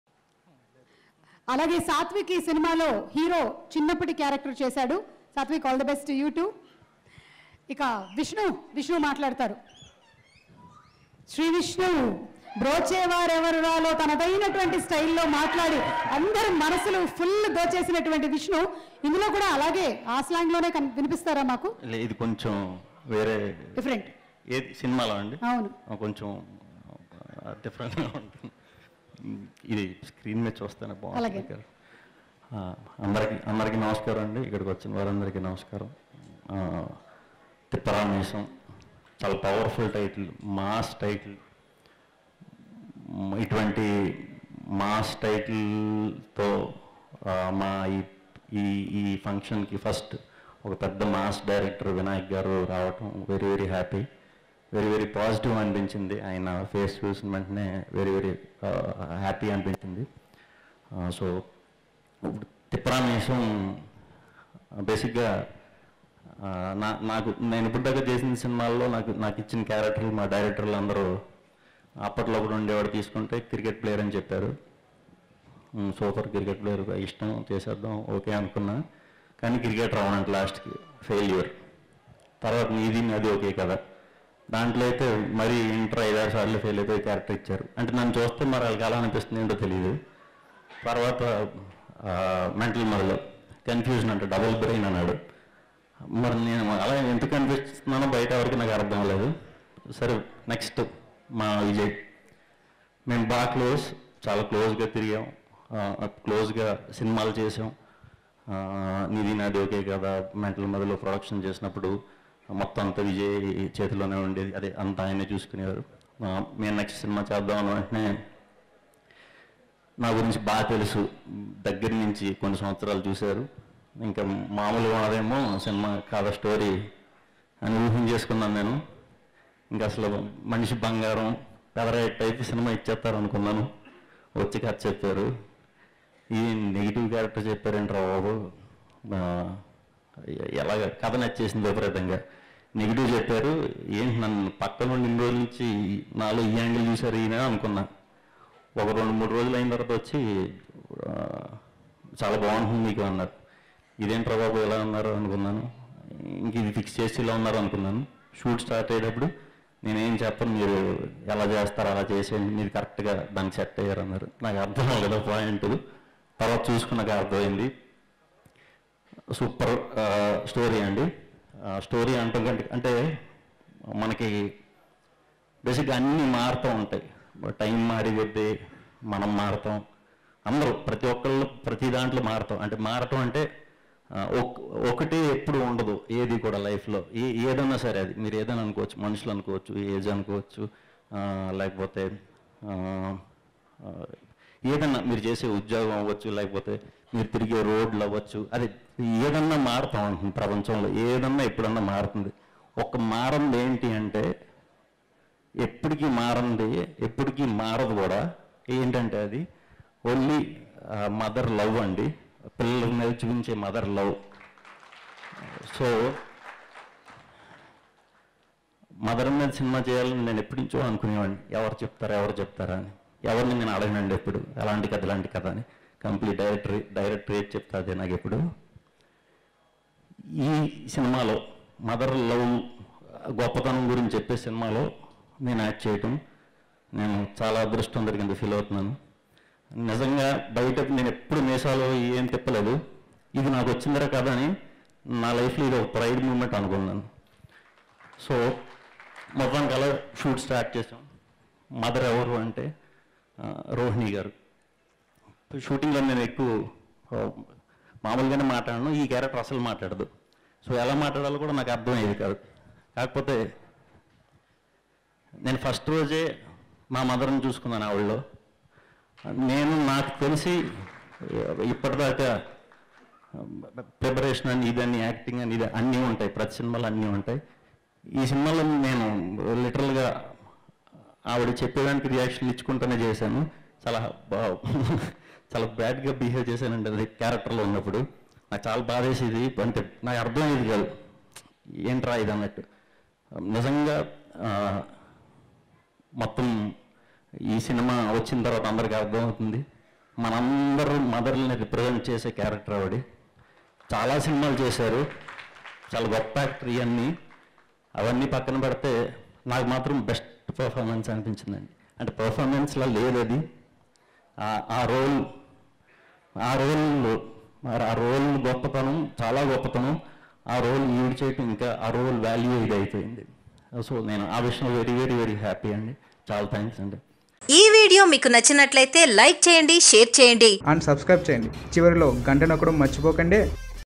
Sri Vishnu Very Emotional Speech About Mother's Love || Thipparaa Meesam Pre Release || LATV
Watch here Sri Vishnu Very Emotional Speech About Mother's Love